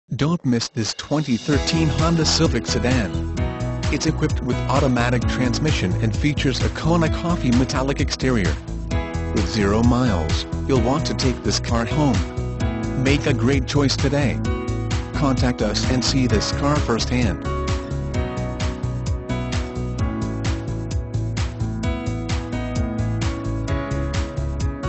2013 HONDA CIVIC SEDAN Webster, TX DE020536
2013 HONDA CIVIC SEDAN Webster, TX
Stock# DE020536

Don't miss this 2013 Honda Civic Sedan. It's equipped with Automatic transmission and features a Kona Coffee Metallic exterior. With 0 miles, you'll want to take this car home. Make a great choice today.